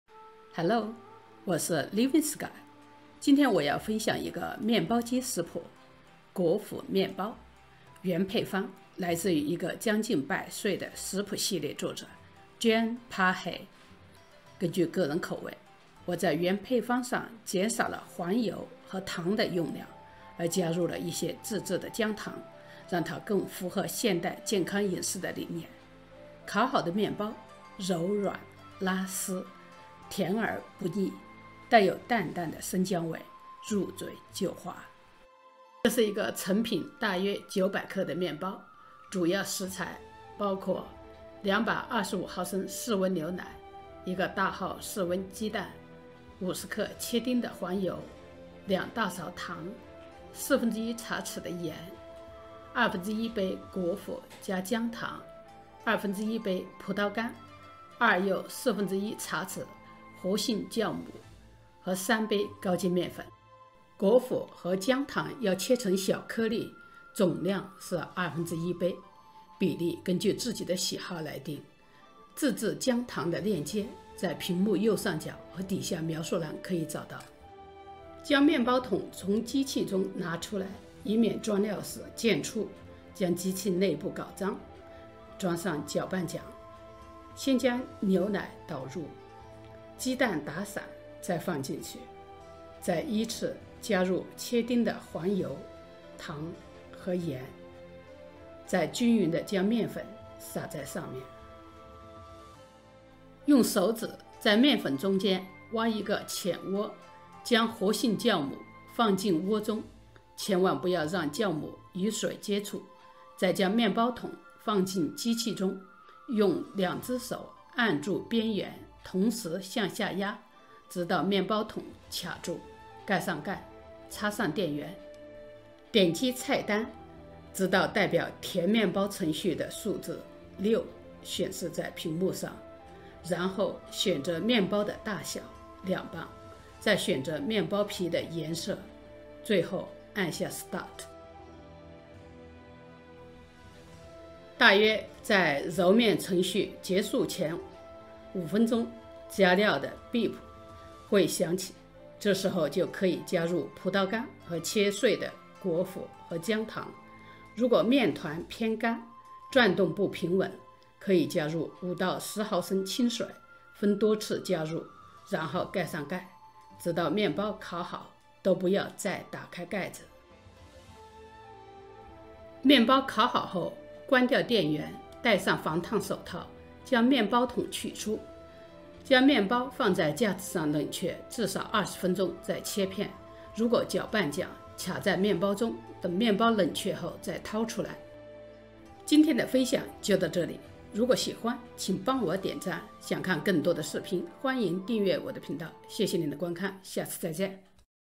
面包机系列食谱之一：果脯面包 / Preserved Fruit Bread | 牛奶鸡蛋和面，低糖少油，柔软拉丝，甜而不腻 | Jean Paré cookbook (V150)
面包机系列食谱之一：果脯面包 / Preserved Fruit Bread | 牛奶鸡蛋和面，低糖少油，柔软拉丝，甜而不腻 | Jean Paré cookbook
今天我要分享一个面包机食谱：果脯面包。原配方来自于一个将近百岁的食谱系列作者Jean Paré 。根据个人口味，我在原配方上减少了黄油和糖用量，而加入了一些自制的姜糖，让它更符合现代健康饮食的理念。烤好的面包柔软拉丝，甜而不腻, 带有淡淡的生姜味，入嘴就化，
这是一个成品大约900克的面包，主要食材包括225毫升室温牛奶，一个大号室温鸡蛋，50克切丁的黄油，2大勺糖，3/4茶匙的盐，1/2杯（果脯+姜糖），1/2杯葡萄干, 2-1/4 茶匙活性酵母和3杯高筋面粉。
果脯和姜糖要切成小颗粒，总量是1/2杯，比例根据自己的喜好来定，自制姜糖的链接在屏幕右上角和底下描述栏可以找到。
将面包桶从机器中拿出来，以免装料时溅出将机器内部搞脏，装上搅拌桨，先将牛奶倒入，鸡蛋打散再放进去，再依次加入切丁的黄油，糖和盐，再均匀的将面粉撒在上面。
用手指在面粉中间挖一个浅窝，将活性酵母放进窝中，千万不要让酵母与水接触，再将面包桶放进机器中，用两只手按住边缘同时向下压，直到面包桶卡住，盖上盖，插上电源。
点击菜单，直到代表甜面包程序的数字“6”显示在屏幕上，然后选择面包的大小：2磅，再选择面包皮的颜色，最后按下“start”。
大约在揉面程序结束前5分钟，加料的Beep声会响起，这时候就可以加入葡萄干和切碎的果脯和姜糖，如果面团偏干，转动不平稳，可以加入5到10毫升清水，分多次加入。然后盖上盖，直到面包烤好，都不要再打开盖子。
面包烤好后，关掉电源，带上防烫手套，将面包桶取出，将面包放在架子上冷却至少20分钟再切片， 如果搅拌桨卡在面包中，等面包冷却后再掏出来。